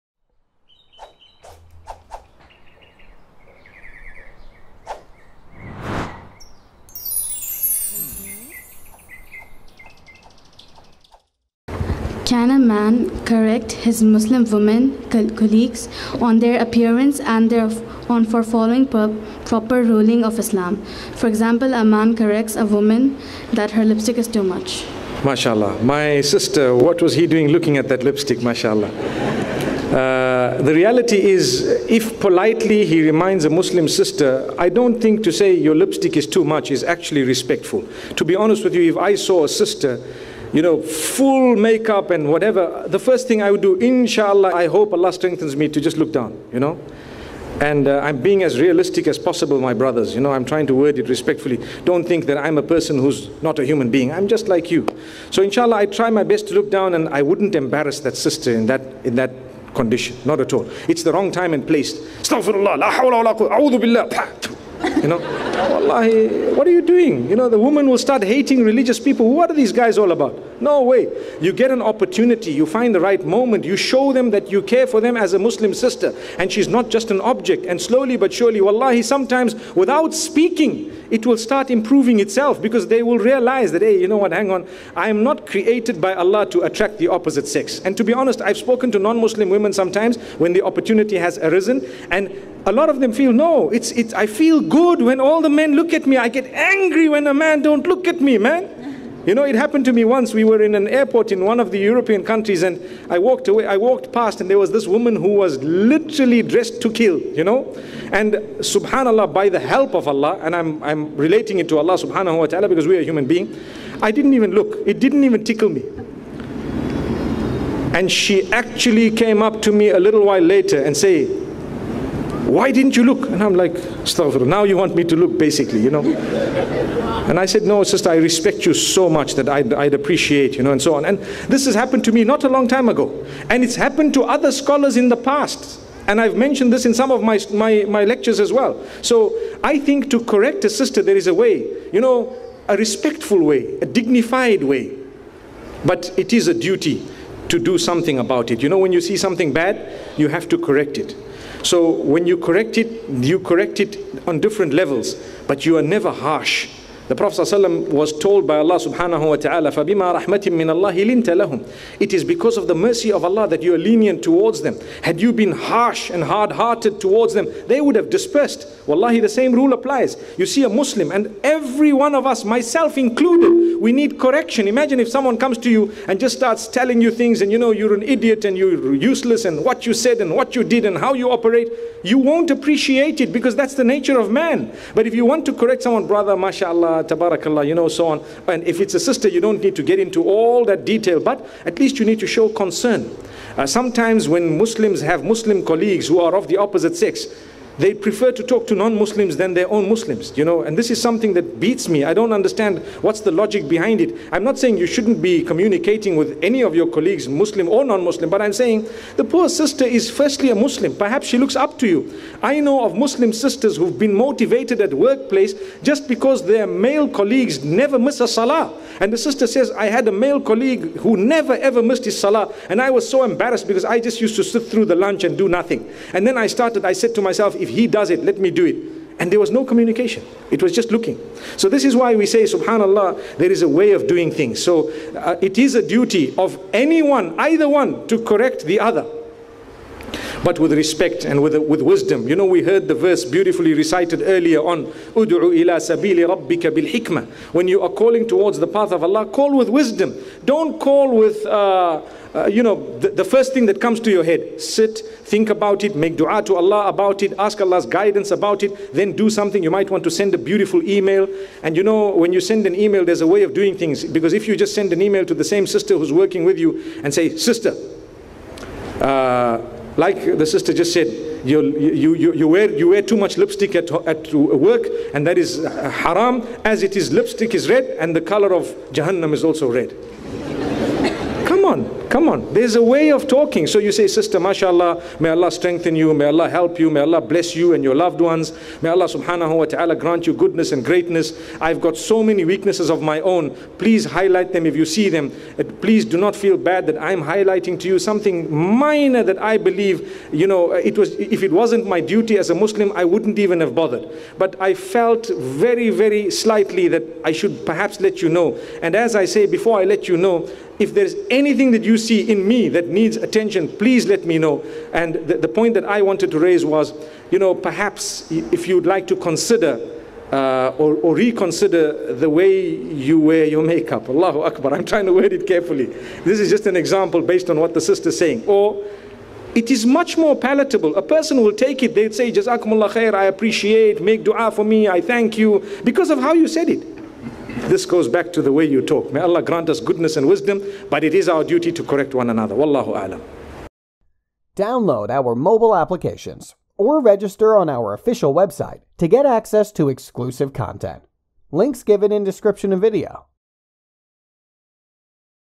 Your Lipstick is Too Much - Mufti Menk
Question: How can we respectfully tell our female colleagues that the make up they are wearing is too much? Can Muslim Woman Wear Make-up?

Answer by Sheikh Mufti Menk - Your Lipstick is Too Much - Blessed Home Series